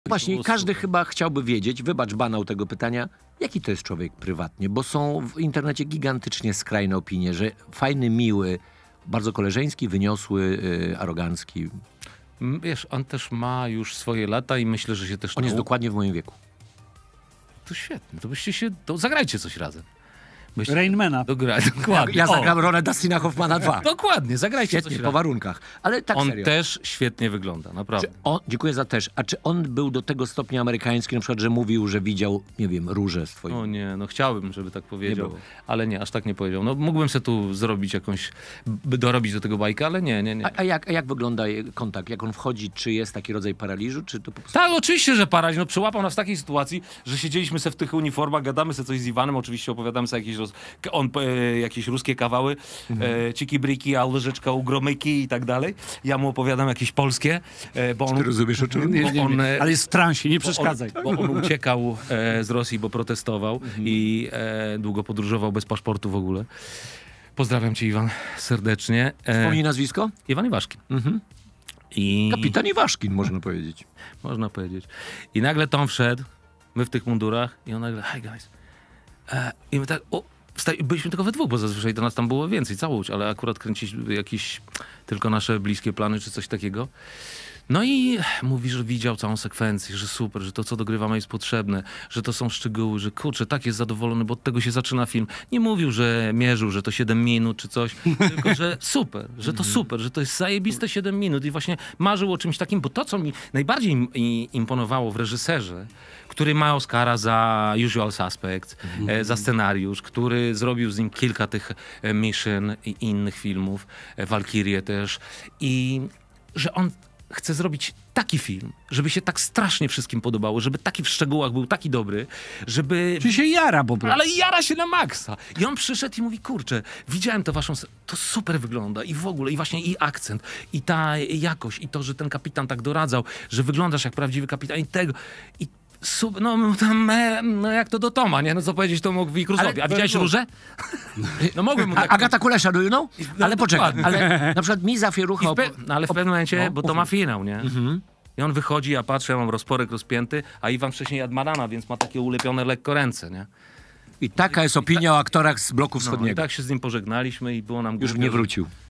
Jakim człowiekiem jest Tom Cruise? | WojewódzkiKędzierski
Gościem podcastu wojewódzkikędzierski był Marcin Dorociński, który opowiedział o swoich doświadczeniach z Tomem Cruisem na planie filowym "Mission: Impossible - Dead reckoning".